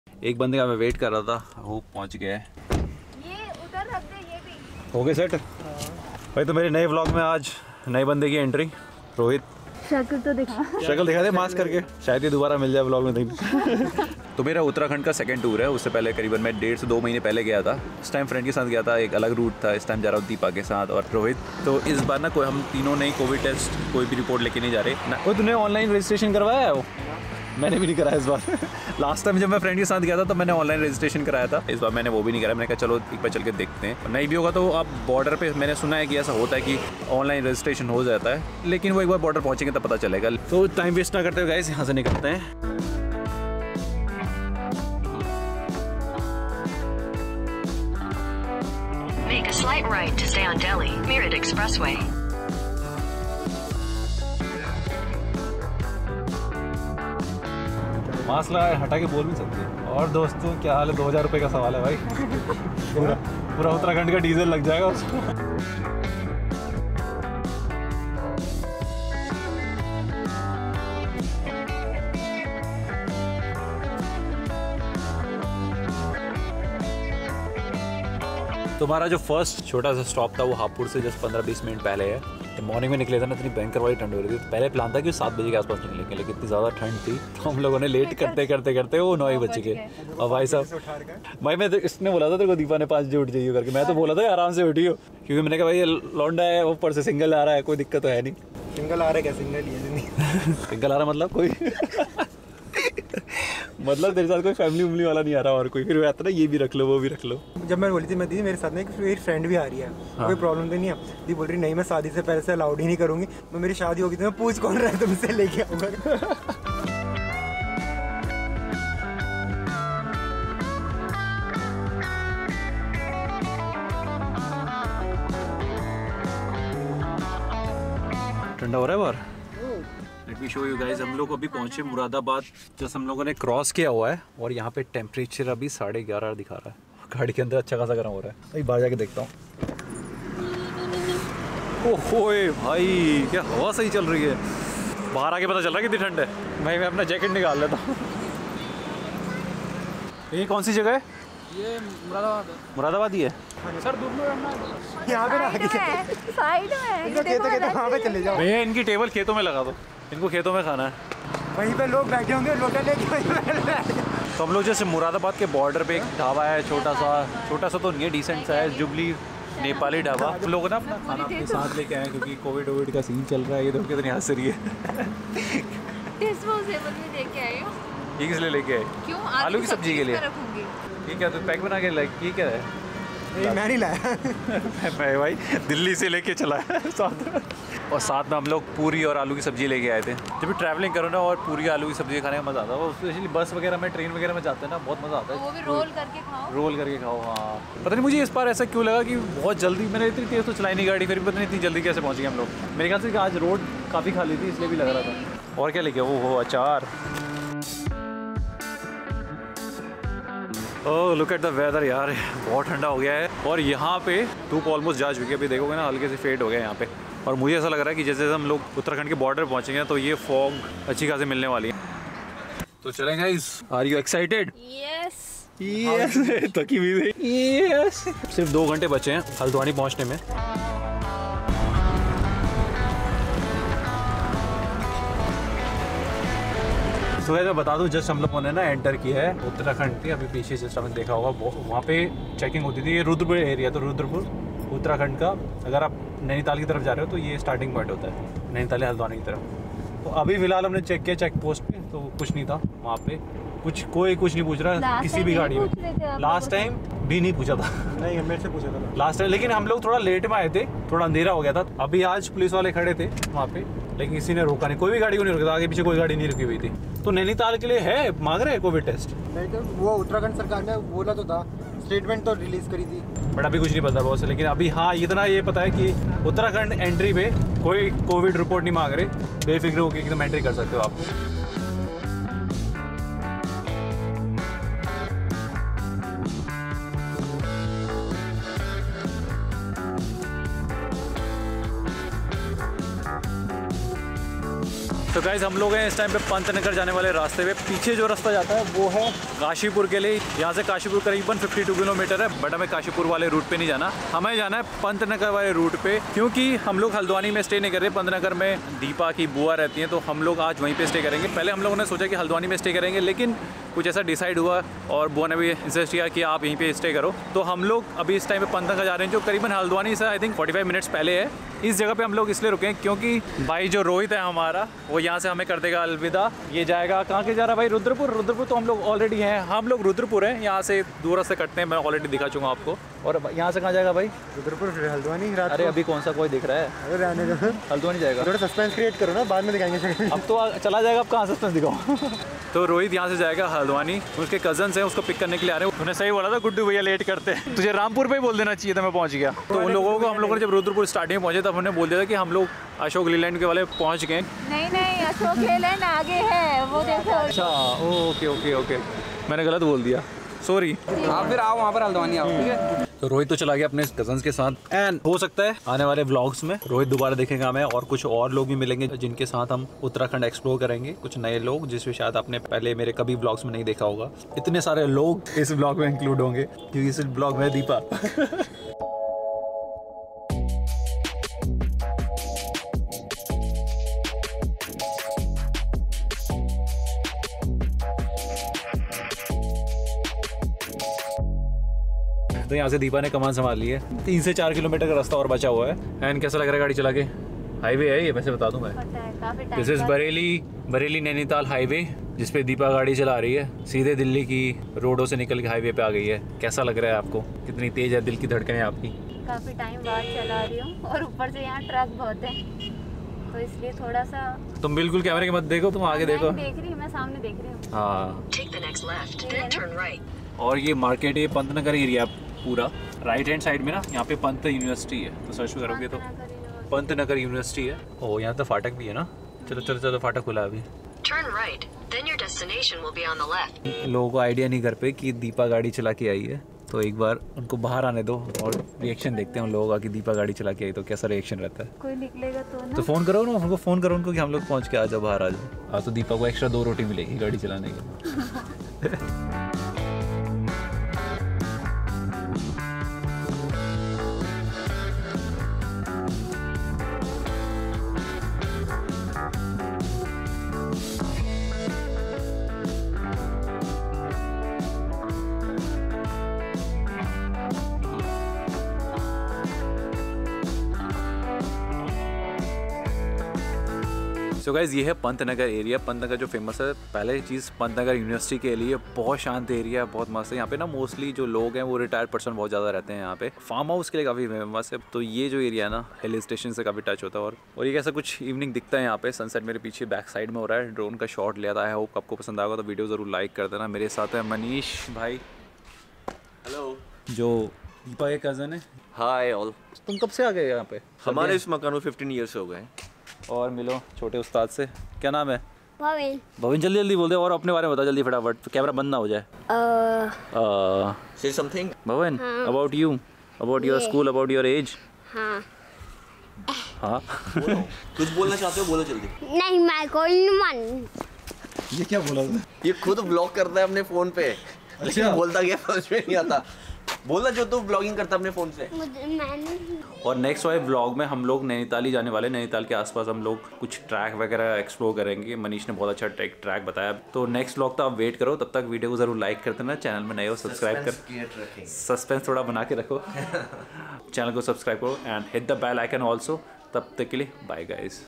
Uttarakhand Road Trip Begins Ep1 | Delhi to Uttarakhand
In this video we are doing another Uttarakhand road trip from Delhi.